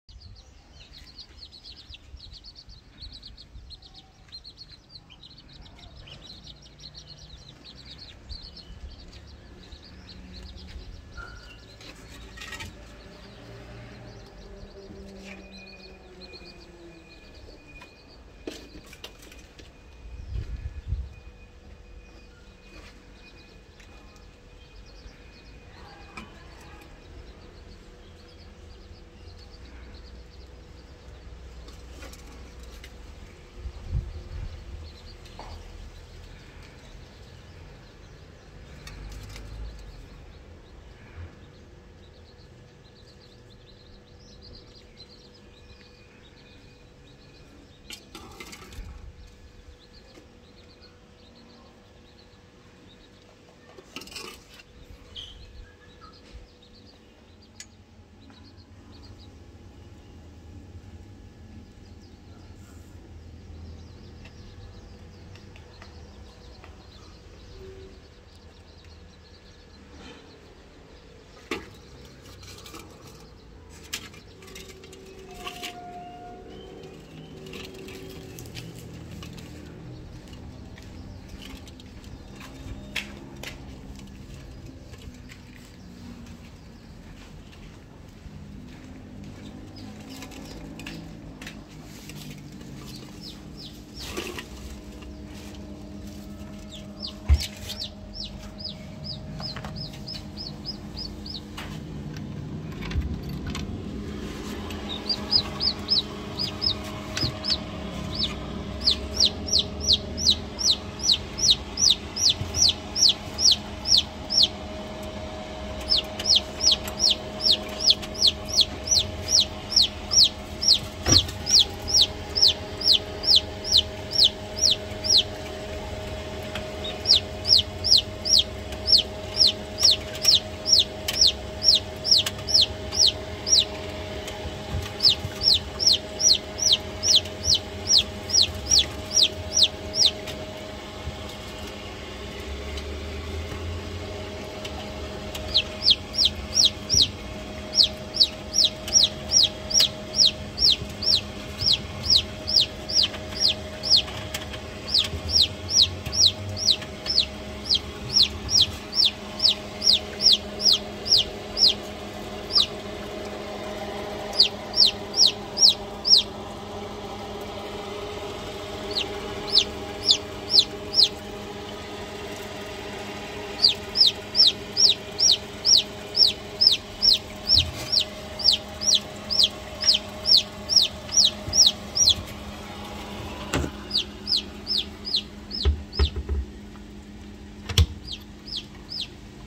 Thu Thập Trứng Gà Chọi Từ Gà Mẹ Mang Đi Ấp Máy-Chờ Đợi 21 Ngày Kết Quả Sẽ Ra Sao
Trứng Gà Chọi